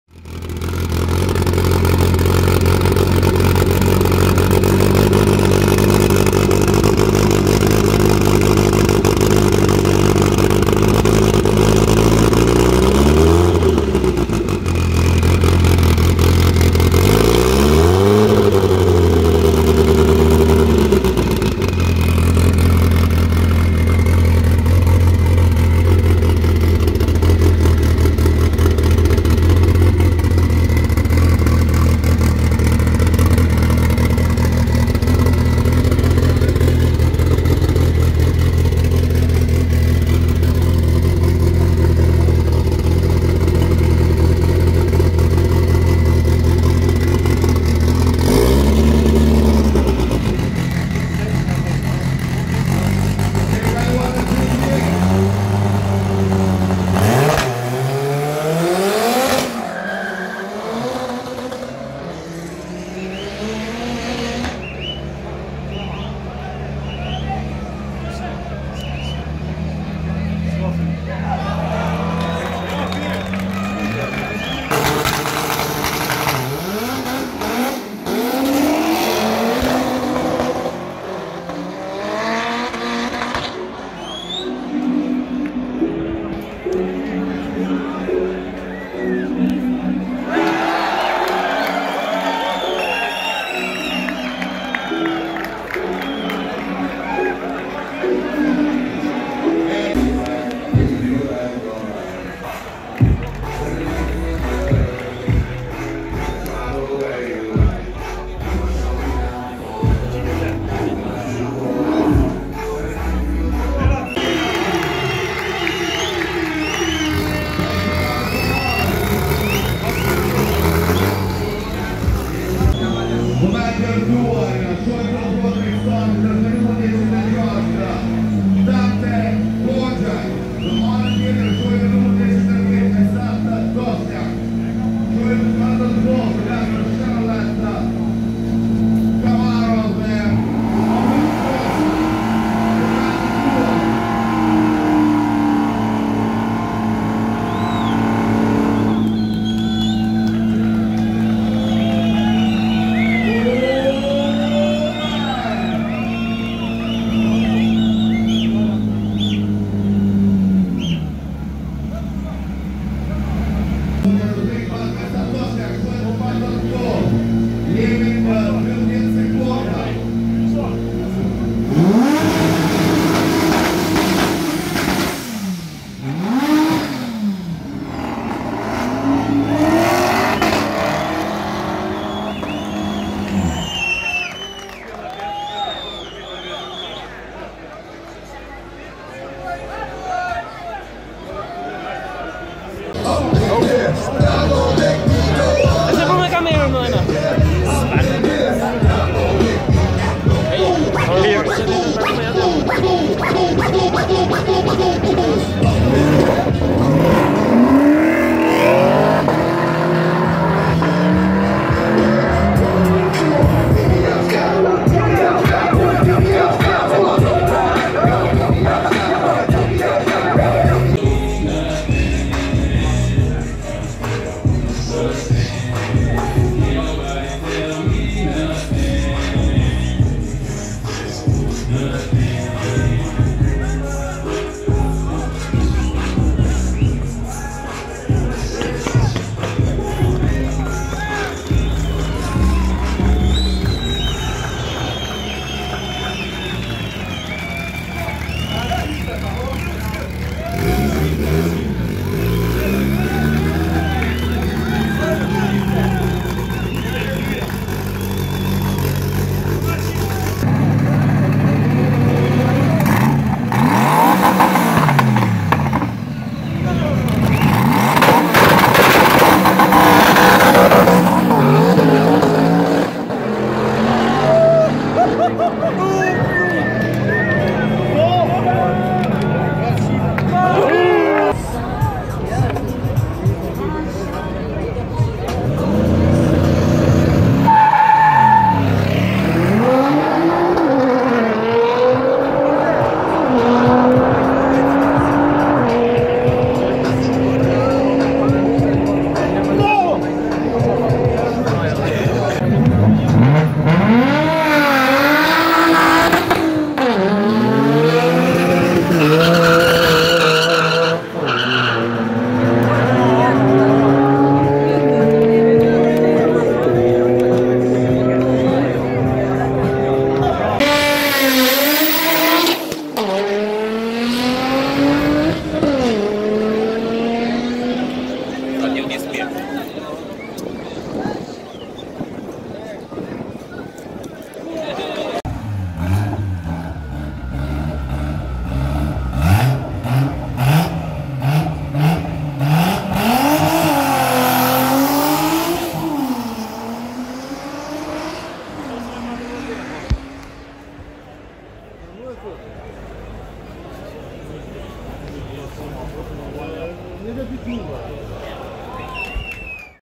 Drag Race Prishtina 3 | 2019 | Highlights
Drag Race Prishtina - 04/08/2019
More than 120 Drivers.
Categories: RWD, FWD, AWD from 16 sec to 8 sec | 1/4 miles.
Overall winner: GOGI Racing Audi S2.
Race organizer: FASK Kosova